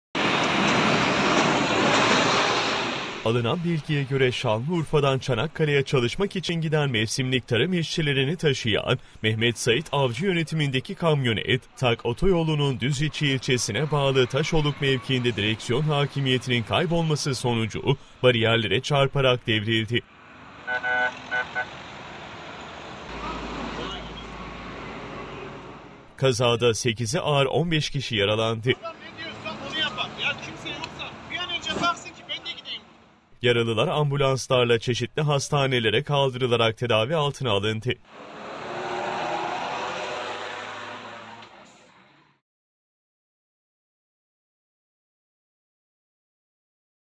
Tarım işçilerini taşıyan kamyonet devrildi: 8’i ağır 15 yaralı
Osmaniye'de Tarsus-Adana-Gaziantep Otoyolunda tarım işçilerini taşıyan kamyonetin bariyerlere çarparak devrilmesi sonucu 8'i ağır 15 kişi yaralandı.